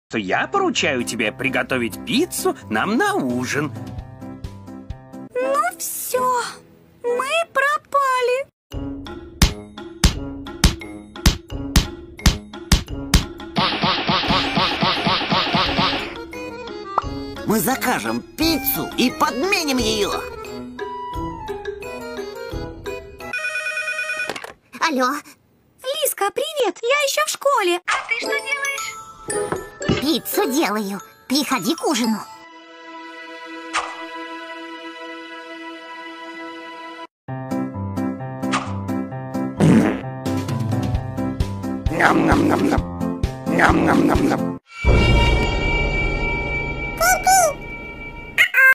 home characters the series episode 1 pizza
cast
Light blue dog as diamond major (20th century fox showtime)
Cat girl as baby scrat (scrat tales)
rooster as katie mitchell (the mitchells vs the machines)
cat as goo goo gaa gaa (20th century fox showtime)